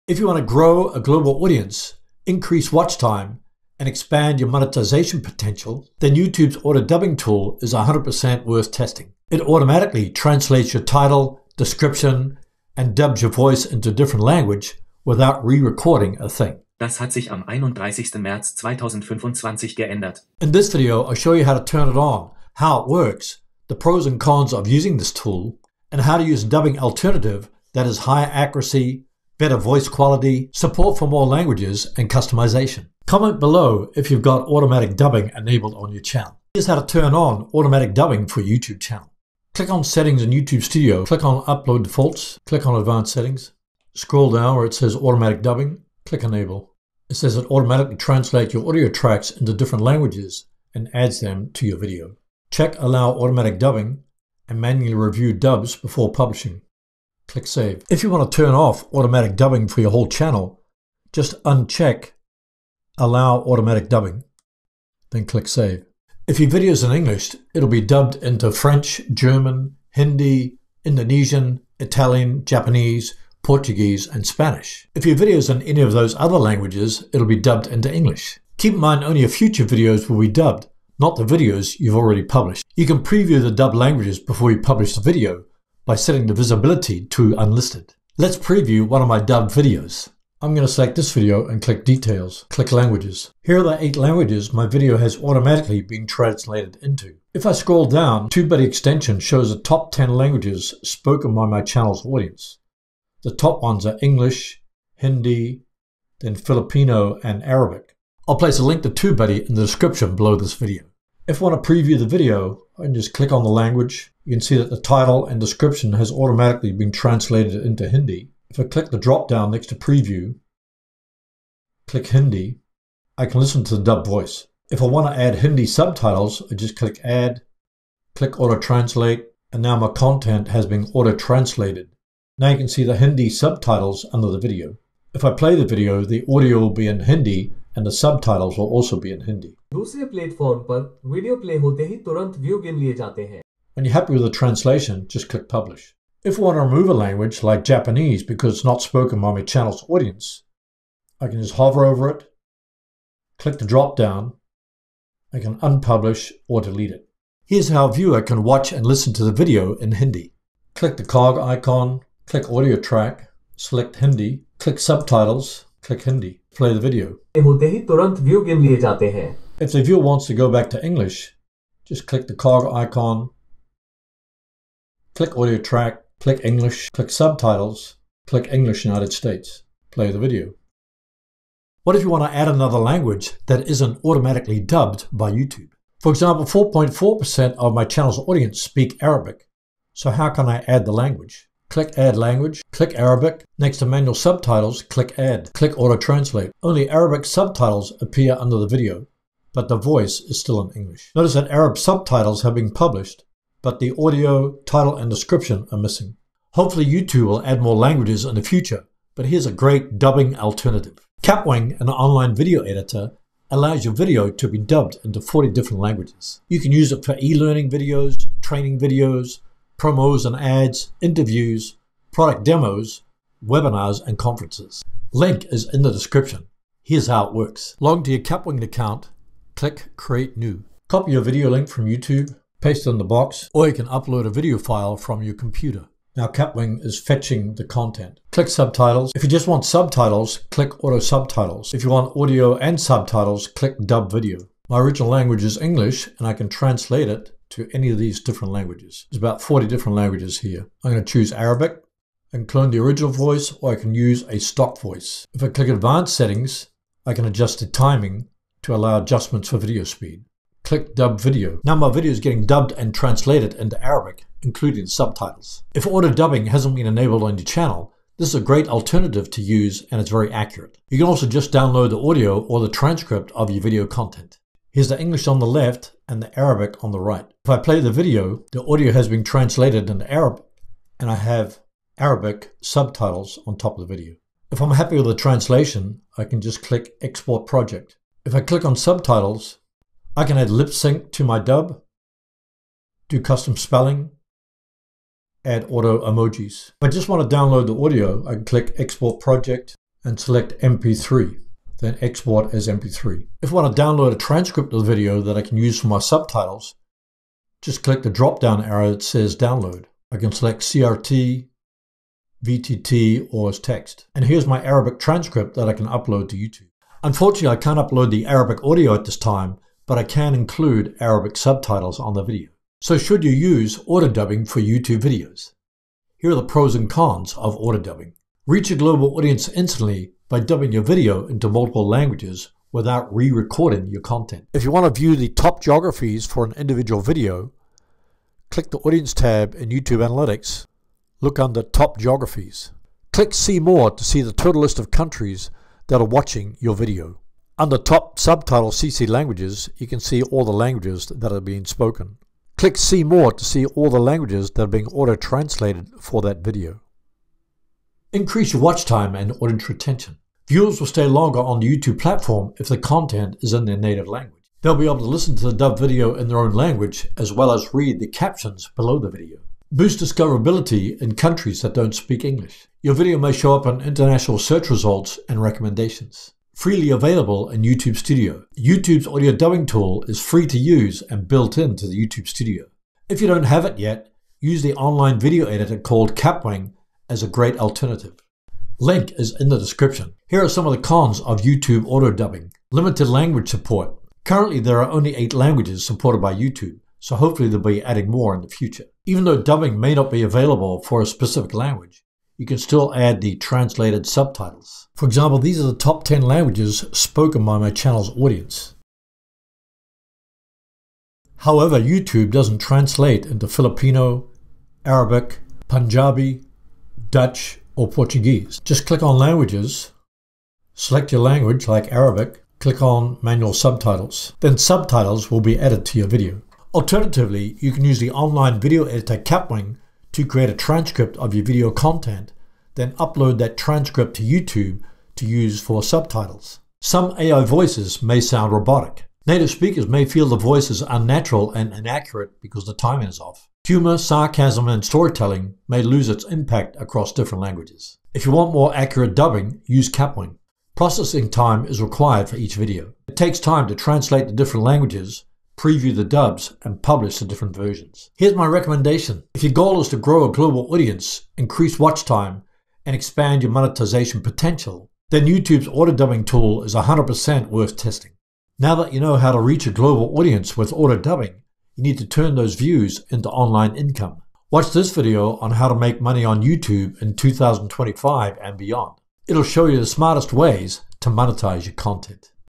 YouTube Auto Dubbing - Reach Millions Without Recording More
Discover how YouTube Auto Dubbing lets you reach millions by automatically translating and dubbing your videos—no extra voiceovers needed! In this video, I’ll show you how it works, how to set it up, and why it’s a game-changer for creators aiming for global reach.